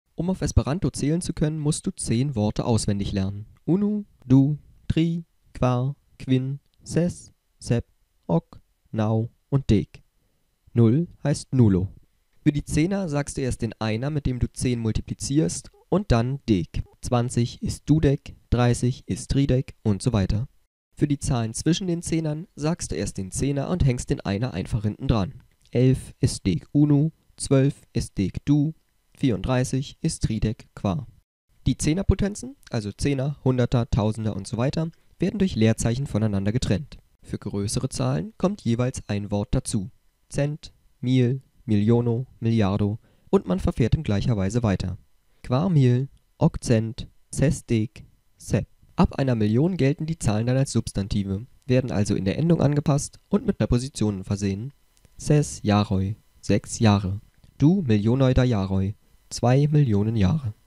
Wie man auf Esperanto zählt
Wie man auf Esperanto zählt - erklärt in einer Minute.